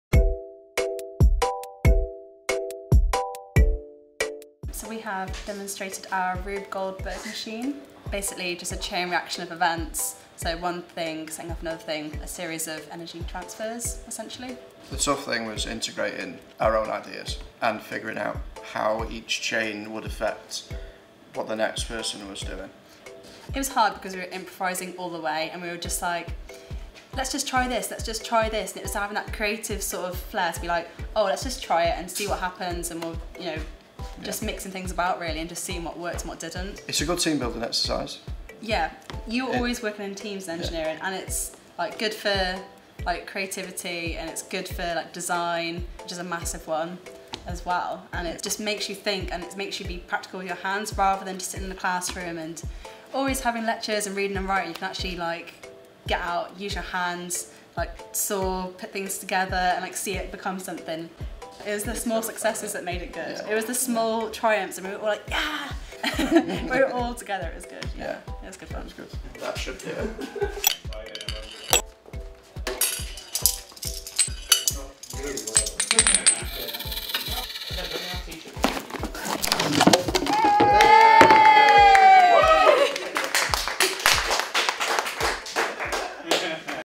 Engineering - Rube Goldberg Machine
Never heard of a Rube Goldberg Machine? Our Engineering students explain how creativity and collaboration helped them build a wonderful and ingenious invention.